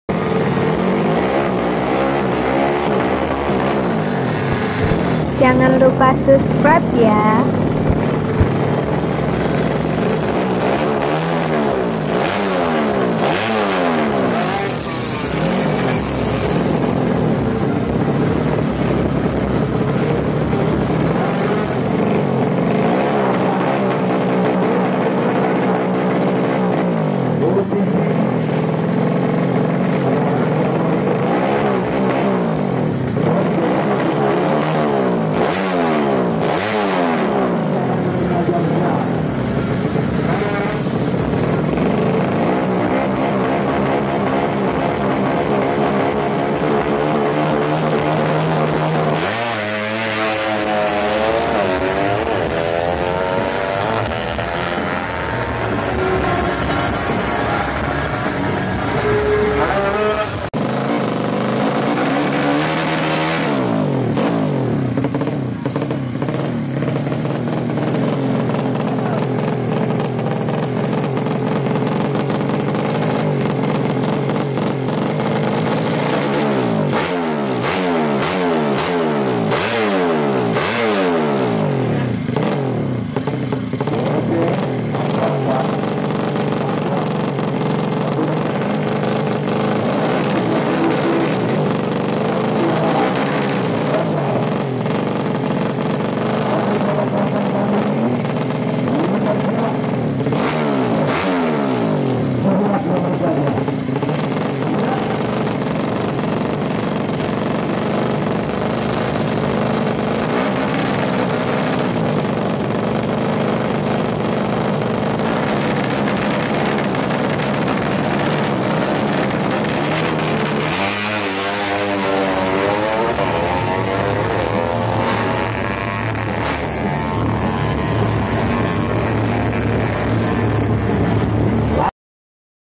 JUPITER Z 130cc Rangkanya Mirip SATRIA FU, drag bike
Drag Bike kejurda jawa timur Ponorogo JUPITER Z 130cc Rangkanya Mirip SATRIA FU
Motor mesin yang digunakan dari Honda, Yamaha, Suzuki dan Kawasaki